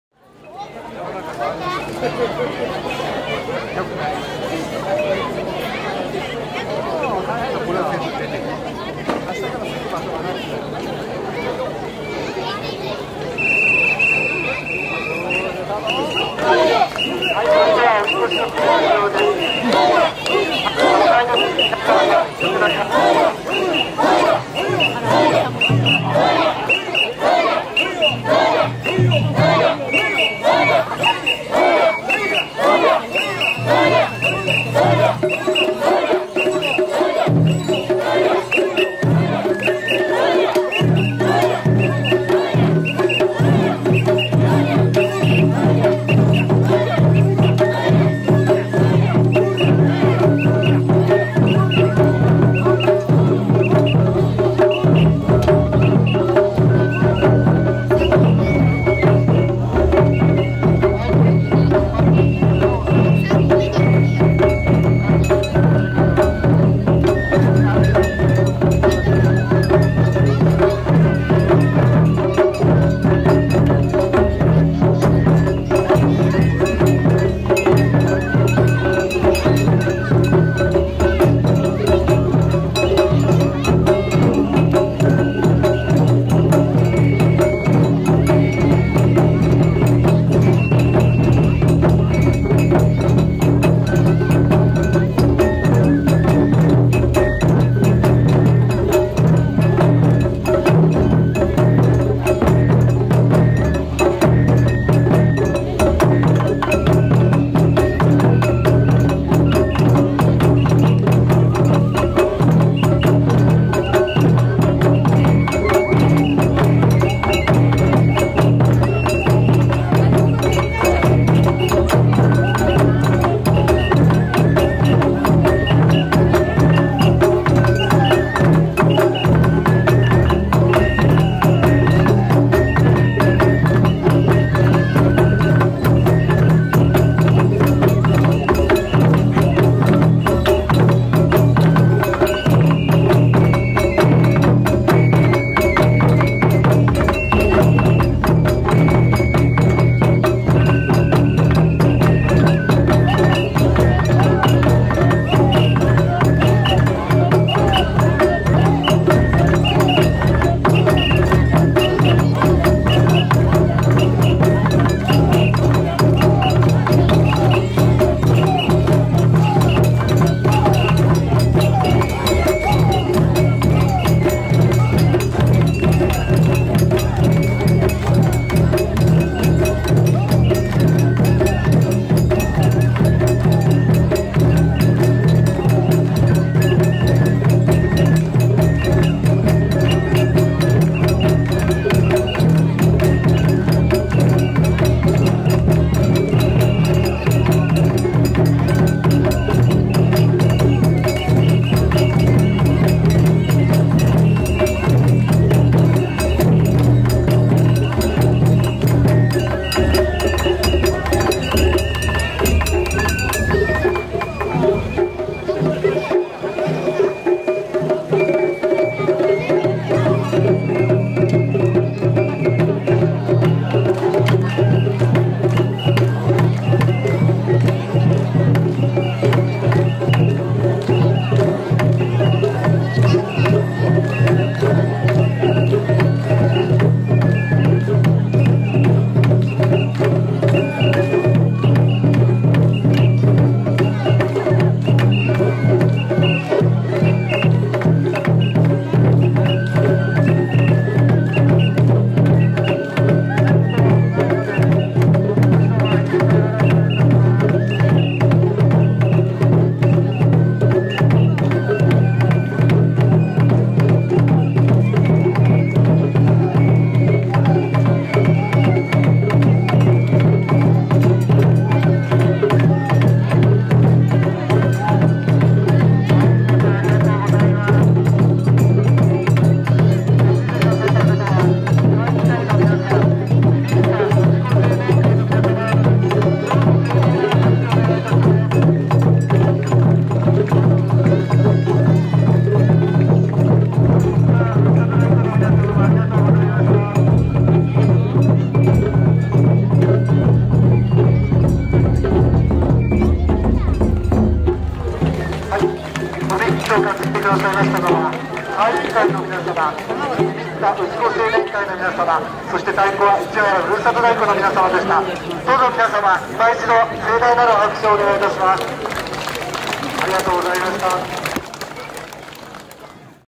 本社神輿 境内 練り歩き！2017年 市原市五井 大宮神社秋季例大祭 宵宮 - Ichihara Goi Omiya Shrine Autumn Grand Festival
毎年11月1日から11月3日の3日間行われる秋季例大祭。
An autumn festival big festival held three days from November 1 to November 3 every year.